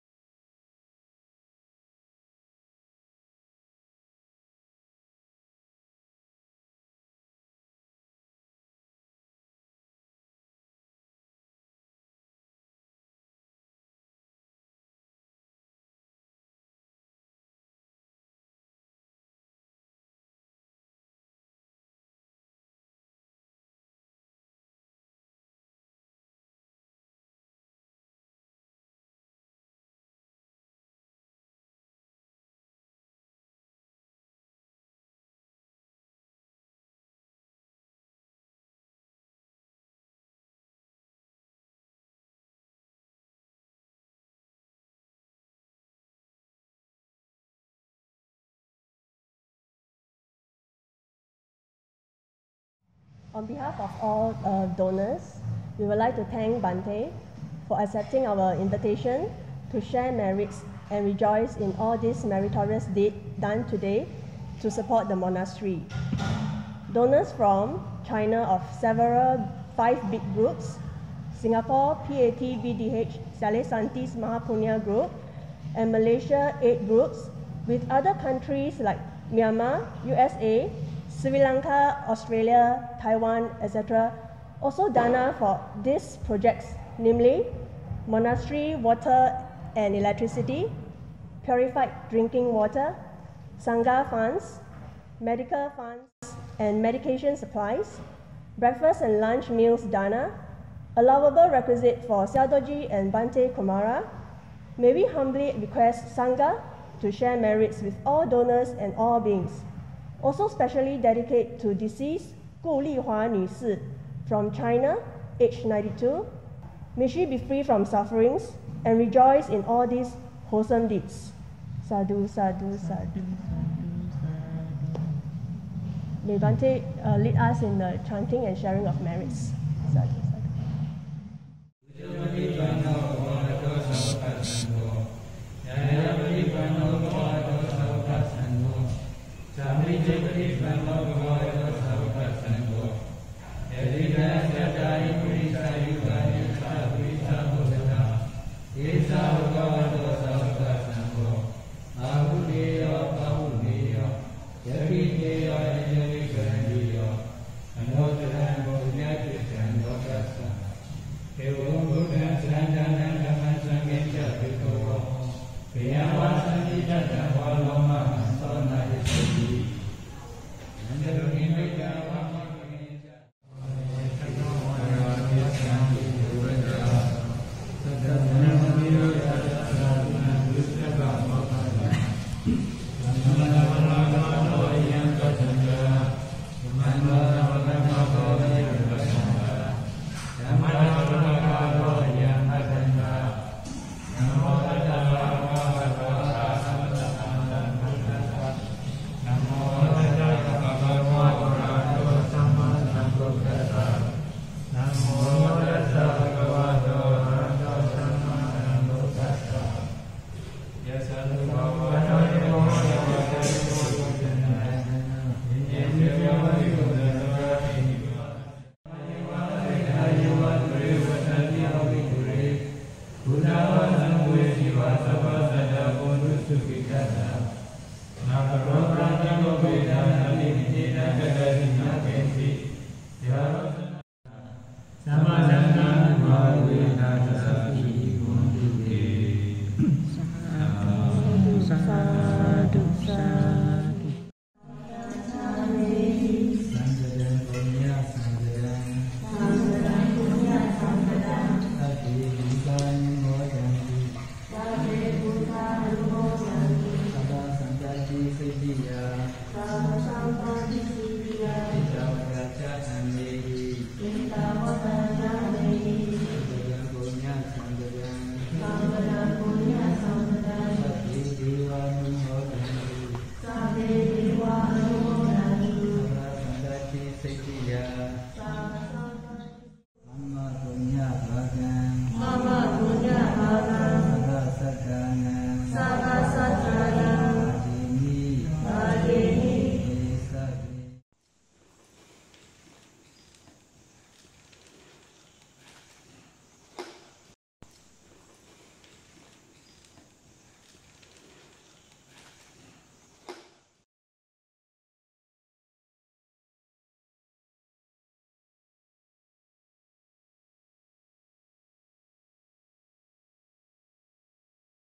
帕奥禅林(彬乌伦) Pa Auk Tawya (Pyin Oo Lwin) | 滴水回向功德 Water Pouring Ceremony | 2 July 2021
Pa Auk Tawya (Pyin Oo Lwin) ~ Water Pouring & Sharing Merits Ceremony

We have respectfully invited Pa Auk Sangha including Sayadaw U Candima, Sayadaw U Kumarabhivamsa and Sayadaw U Janakabhivamsa for a water pouring ceremony.

Sayalay Jhanaparami (China) and Sayalay Paññasāra (Singapore) at Pa Auk-Pyin Oo Lwin will represent all donors to do the water pouring for merits sharing. May all donors and your families rejoice in this merits sharing. Sādhu! Sādhu! Sādhu!

May all beings be free from danger!
May all beings be free from mental and physical suffering!
May all beings be well, healthy and happy!
May all wholesome wishes be fulfilled always!
May the merit Dana, Sila, Bhavana be the supporting condition for Nibbāna!
Buddhasāsanaṁ ciraṁ tiṭṭhatu!
Sādhu! Sādhu! Sādhu！

帕奥禅林(彬乌伦) ~ 滴水回向功德

我们恭敬地邀请了帕奥僧团包括乌参迪玛西亚多、乌库玛拉毗旺萨西亚多和迦那卡毗旺萨西亚多给大家滴水回向功德。

在禅林的巴拉蜜尼师Sayalay Jhānapārami（中国）和慧涵尼师Sayalay Paññasāra（新加坡）将代表所有的布施者进行滴水回向功德。愿所有布施者和您们的家人都即时随喜功德分享！Sādhu! Sādhu! Sādhu!

愿一切众生没有危险！
愿一切众生没有身心的痛苦！
愿一切众生平安、健康、快乐、吉祥！
愿一切善愿都能成就！
愿大家所累积的布施、持戒和禅修功德成为早日证悟涅槃的助缘！
愿正法久住！
随喜！随喜！随喜！